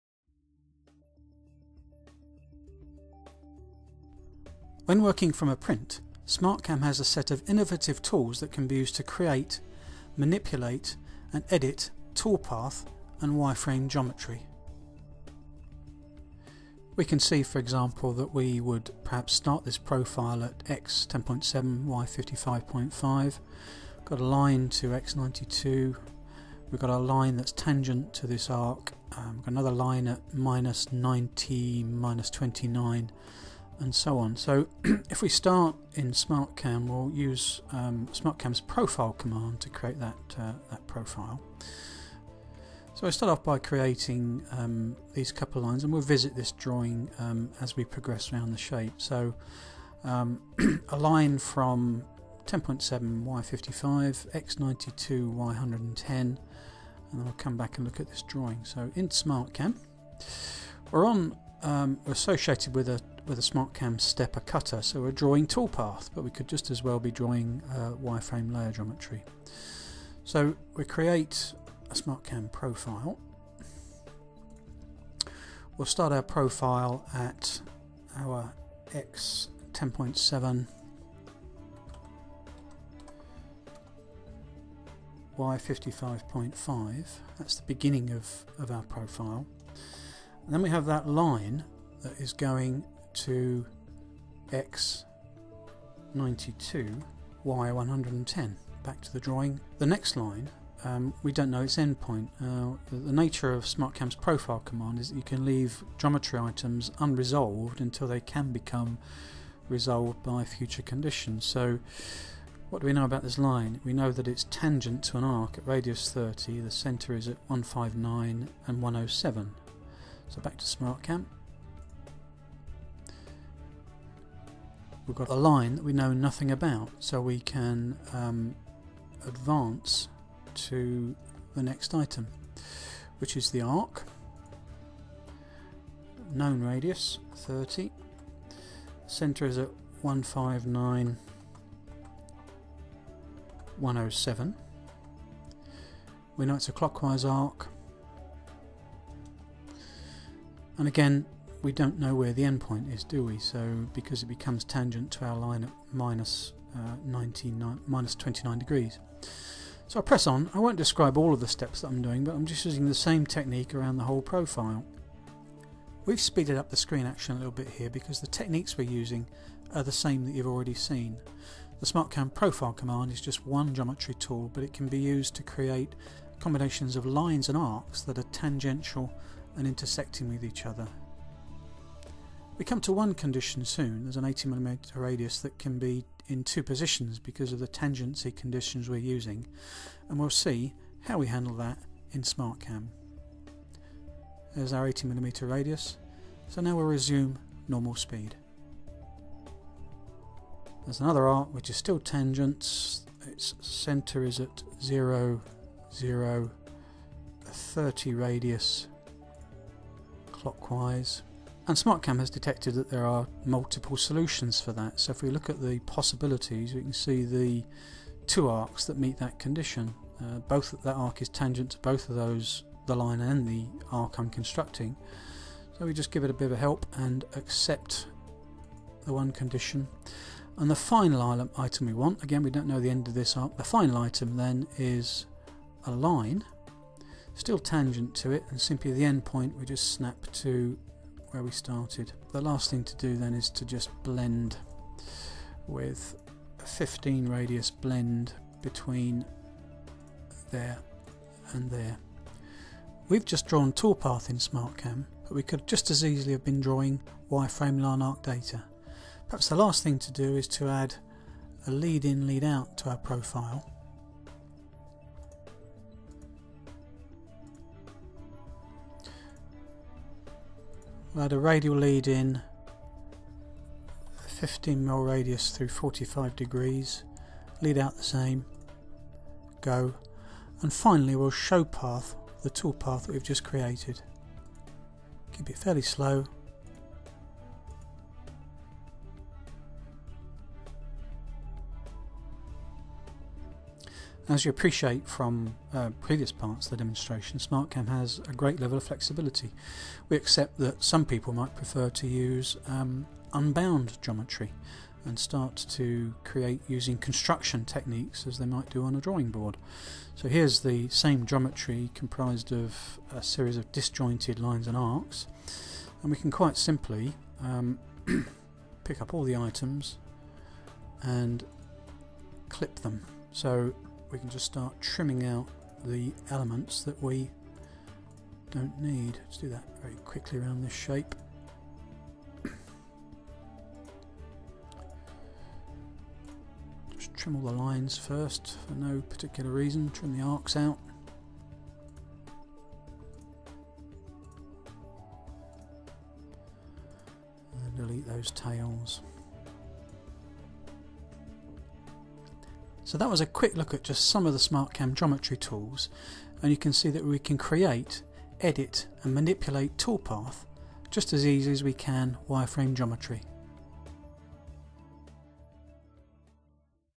13 - Constructing Geometry in SmartCAM
Part 13 of 18 in a short series of videos introducing SmartCAM Production Milling, the 2-2½ axis CAD/CAM software solution for CNC machining.